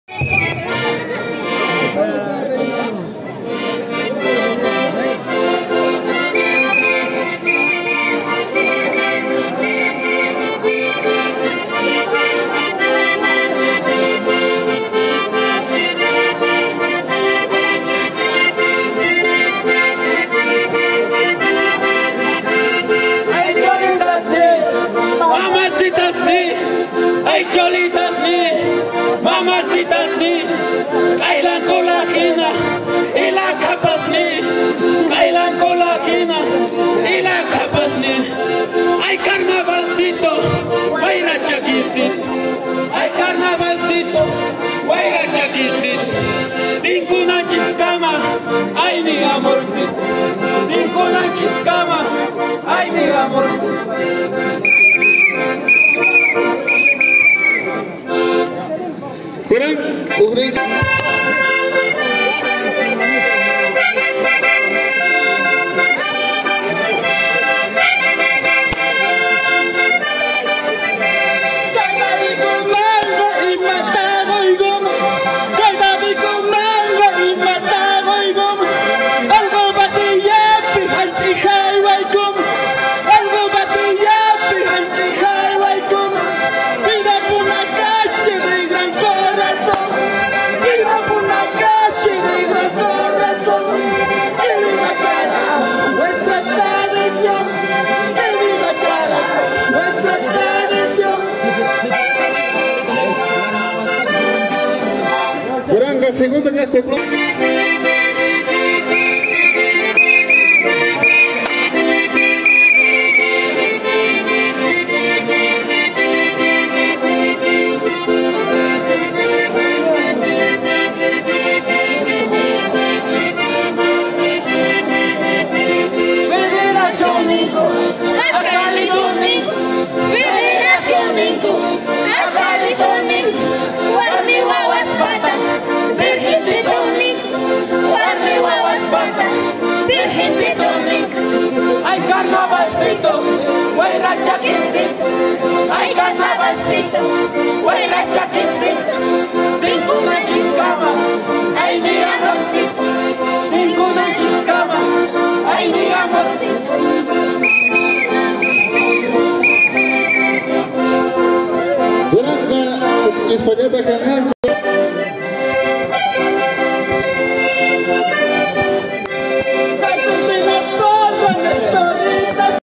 CARNAVAL 2022 EN LA COMUNIDAD PUNACACHI DE MUNICIPIO MOROCHATA PROVINCIA AYOPAYA_ COCHABAMBA_BOLIVIA
Concurso de Takipayanakus que se realizó en diferentes comunidades de  Municipio  Morocha  Provincia Ayopaya  del depto. Cochabamba_Bolivia.